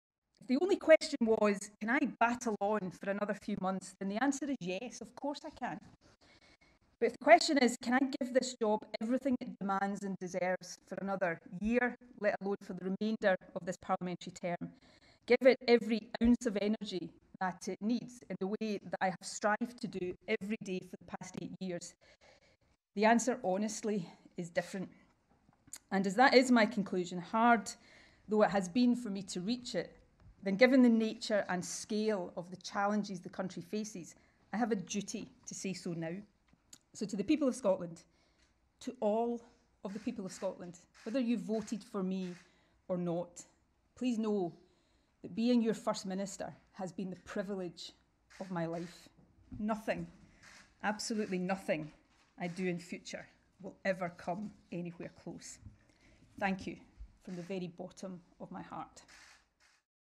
Nicola Sturgeon explains her reasons for standing down as First Minister
During a press conference at Bute House, Nicola Sturgeon explained why she has made the decision to step down as First Minister